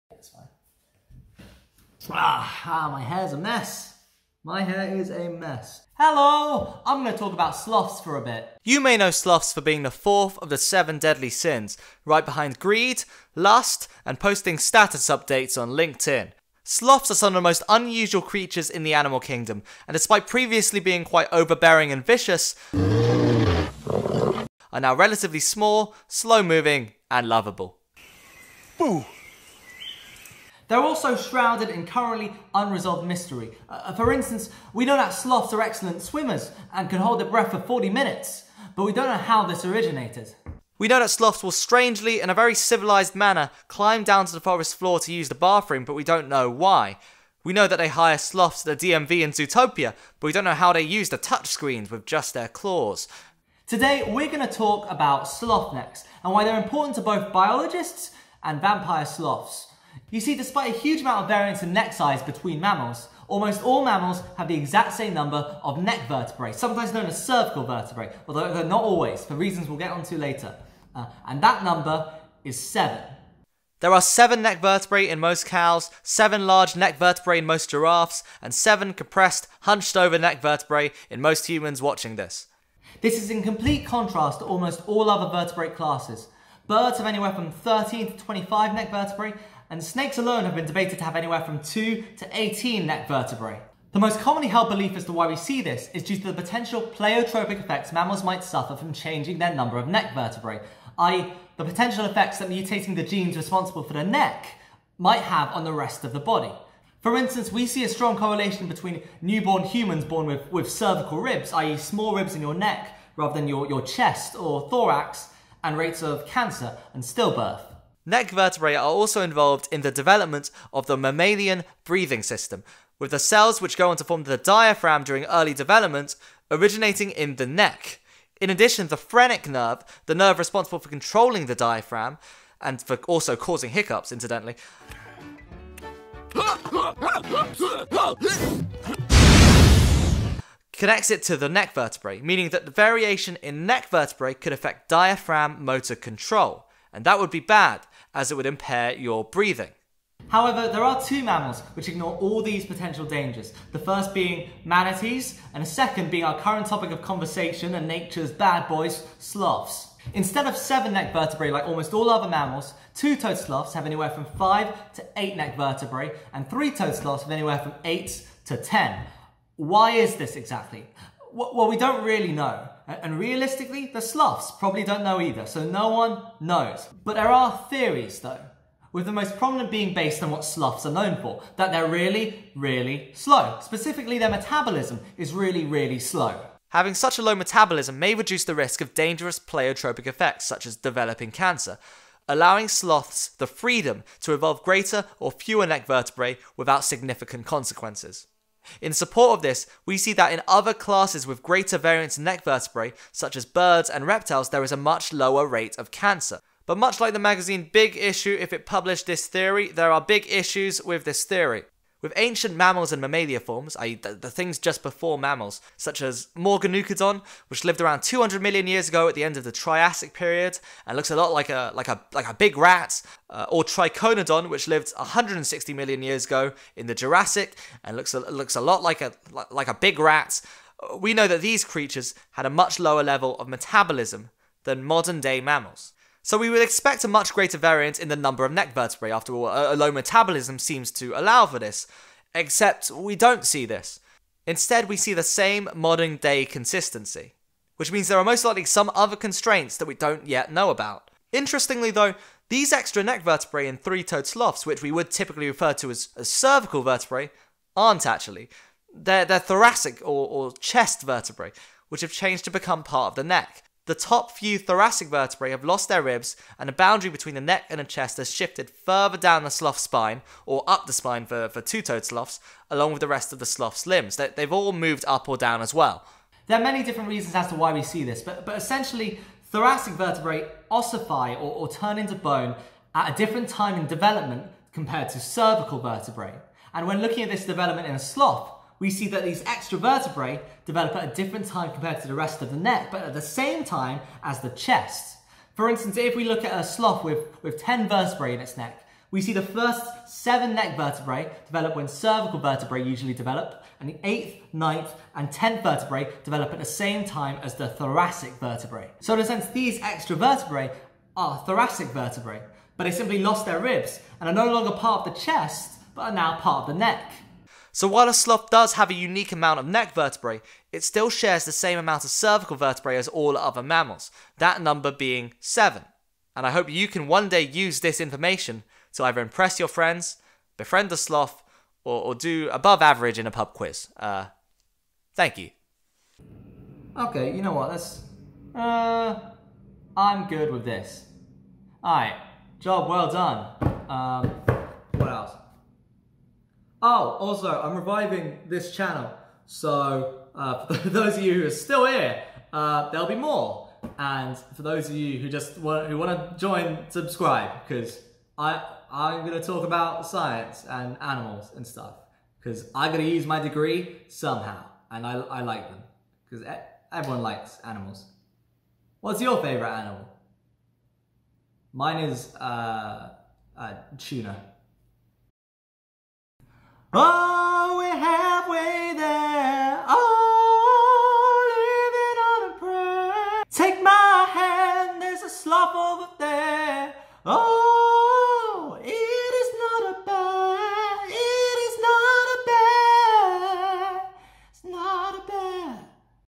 Why Do Sloths have Strange Necks?
Hello! I'm back! And talking about mammal necks, sloths, extra vertebrae, and soulful ballads.